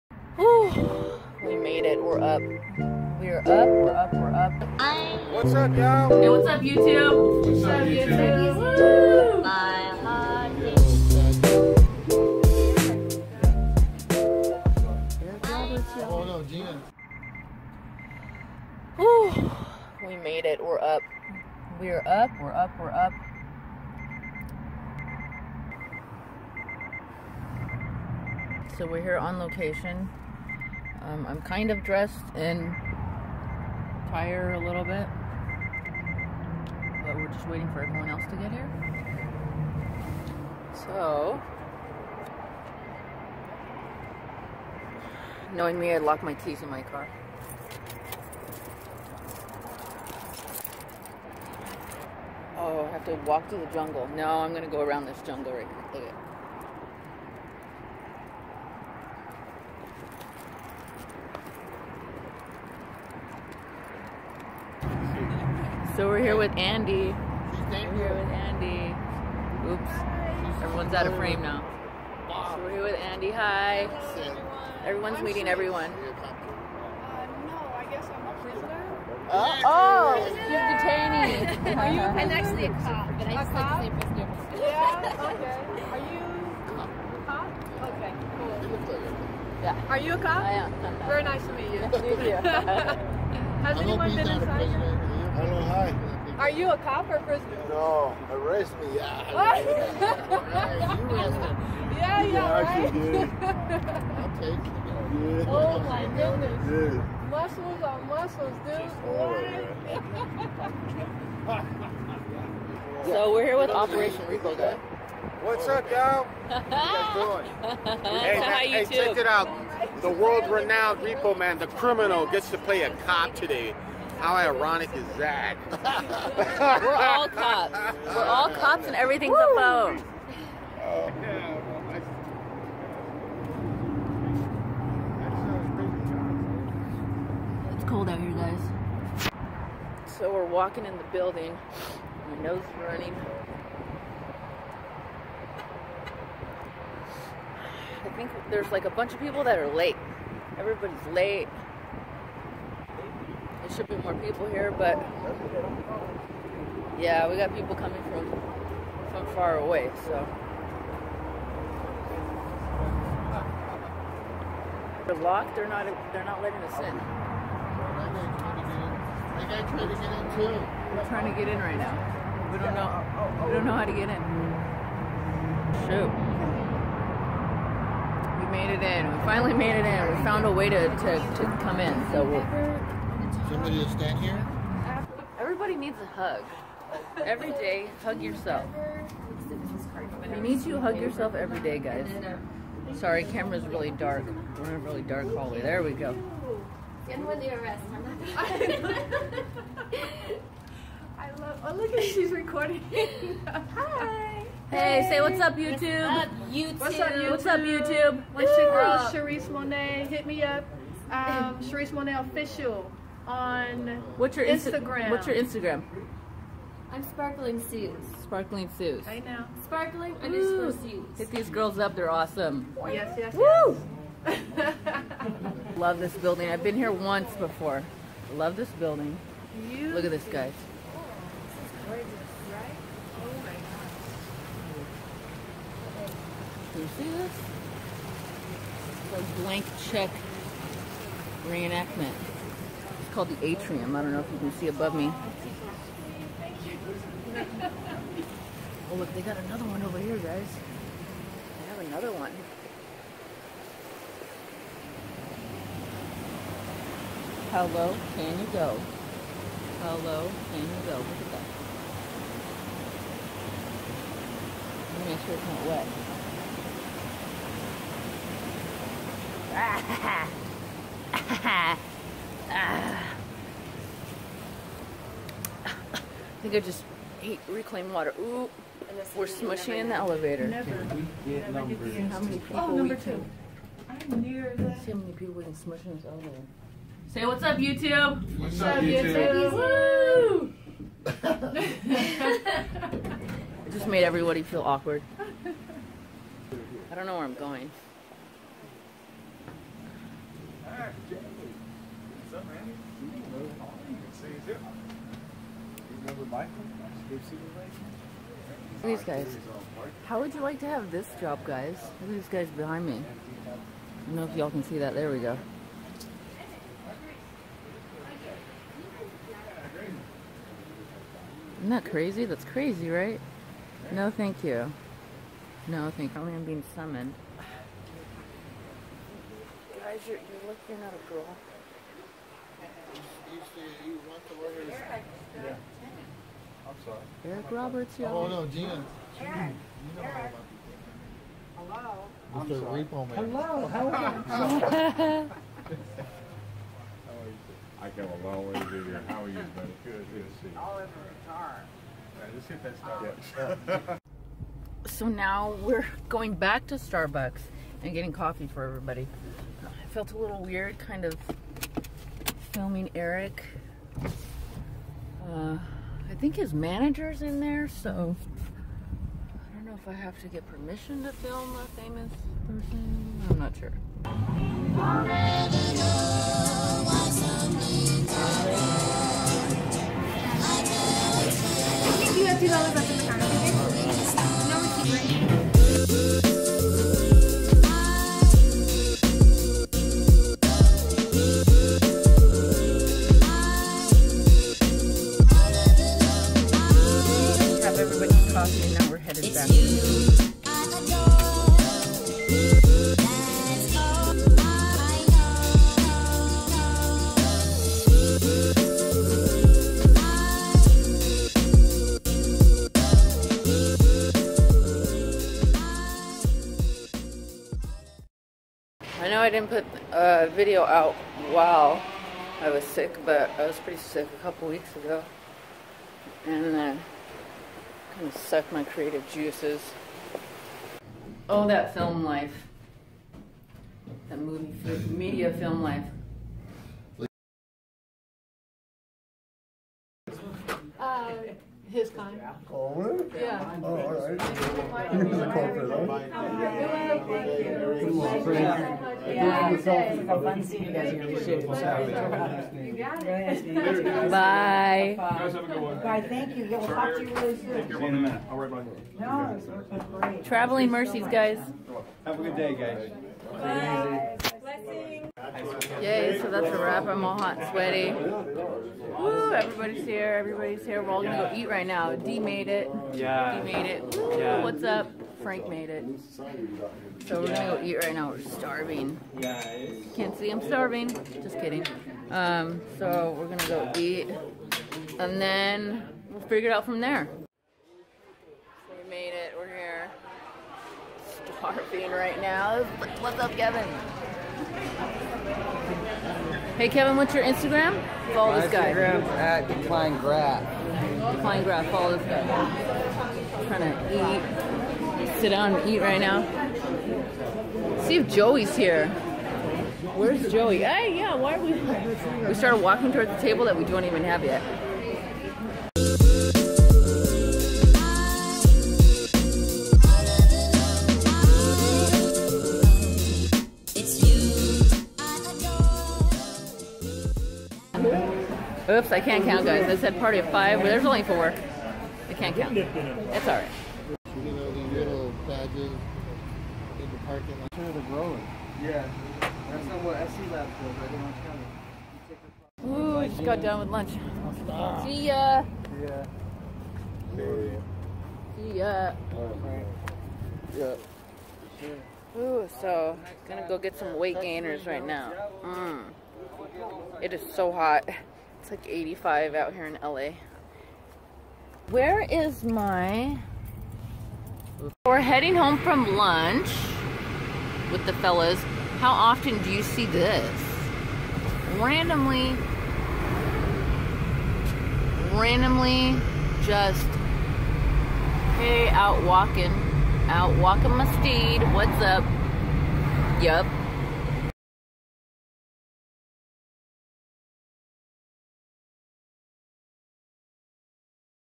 FILMING WITH ERIC ROBERTS
Had a blast filming with the famous Eric Roberts such a humbling experience to act in scenes with him. Hope to see more of him someday.

MY GEAR:

Helmet : HJC

Icon Gloves

AplineStar boots

Camera: Contour PLUS2

Contour ROAM 1600

Camera Helmet Mount